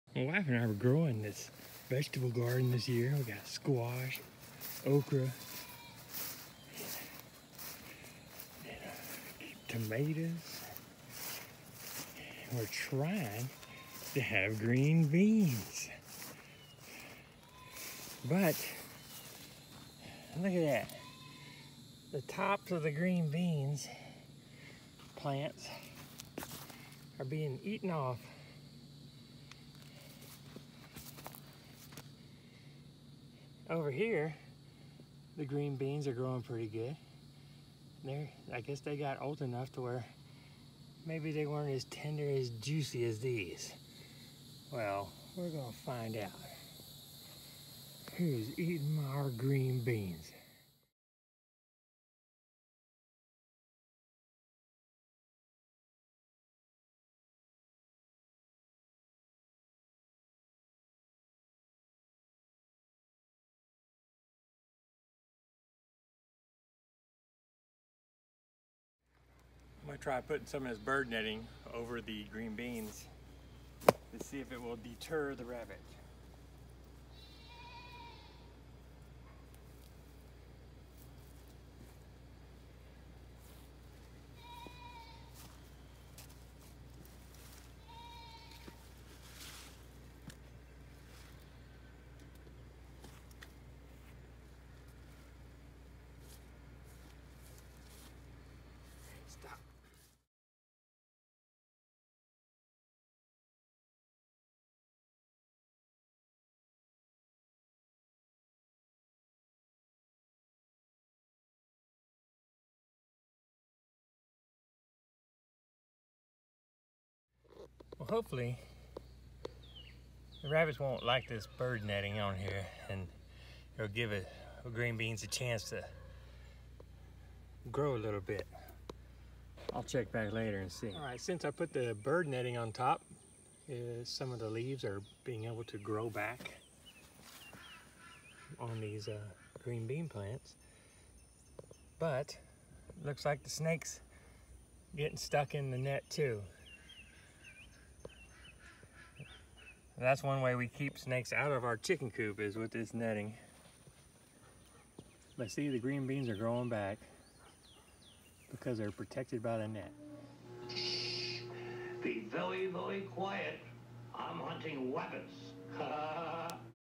Protect your garden from rabbits with bird netting
Rabbits have been eating our young green been plants. I put 1/2” mesh bird netting over the plants to hopefully protect them from the furry bean eating monsters.
bird netting: